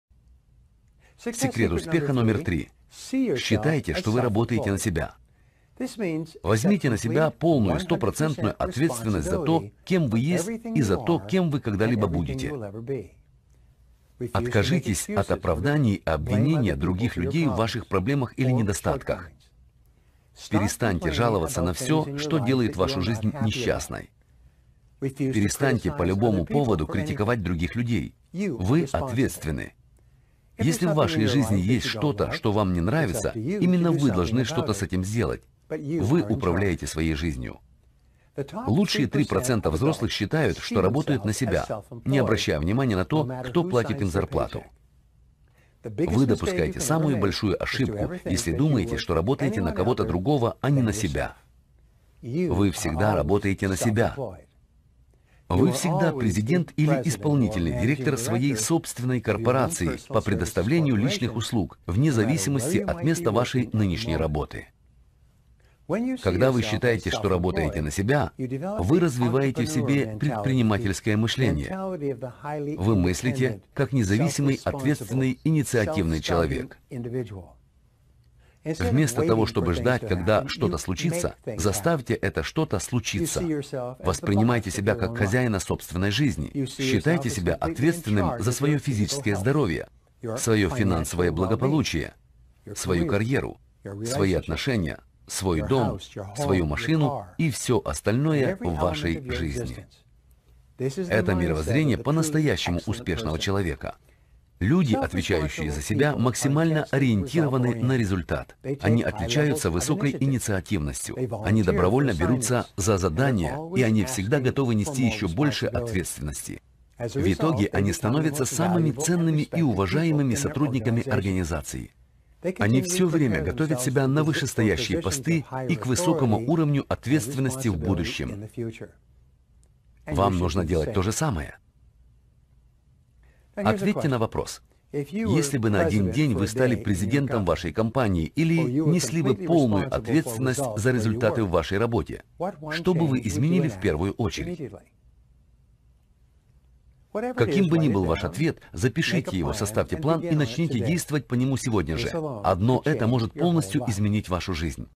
Секрет успеха №3 из 21 секретов успеха миллионеров от Б.Трейси
В этом видео ролике вы увидите, как один из лучших бизнес тренеров мирового значения Брайн Трейси делится третьим из 21 секретов миллионеров. Из этого ролика узнаете, что каждый может добиться успеха, может стать миллионером, проделывая определенные действия.